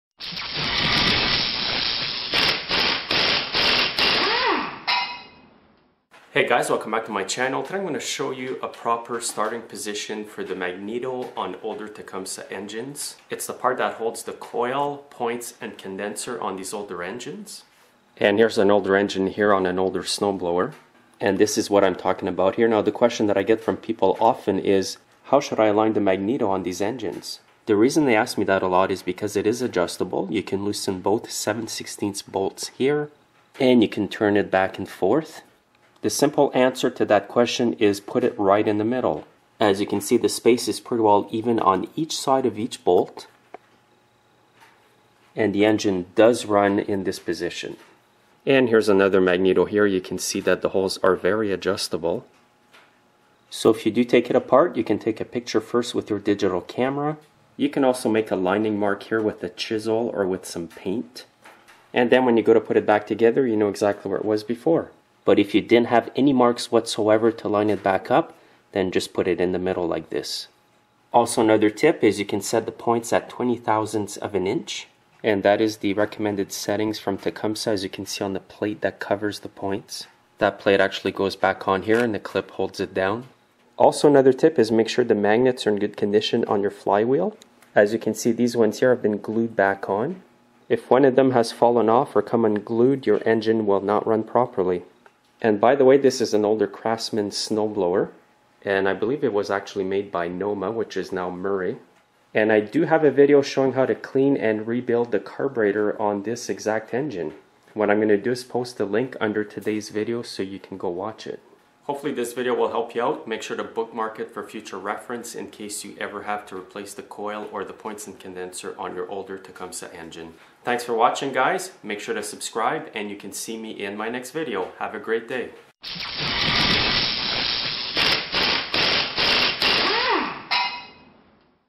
Tecumseh Ignition Magneto Setting & Tips
Ever wonder what a good setting is for the magneto on your older Tecumseh engine is? In this video I show you a good setting to ensure your engine will start.

This is on engines with points and condenser and coil located under the flywheel

Watch "How to clean the carburetor" videos on this same engine;